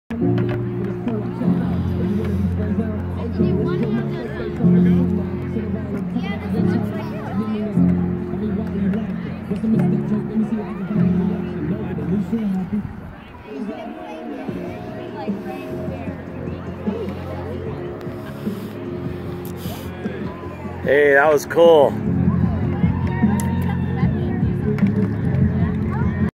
Emery & Talon make HUGE Soap Bubbles Epilepsy 5k
Emery & Talon make HUGE Soap Bubbles at the Epilepsy 5k in Phoenix, AZ on Saturday, March 24, 2018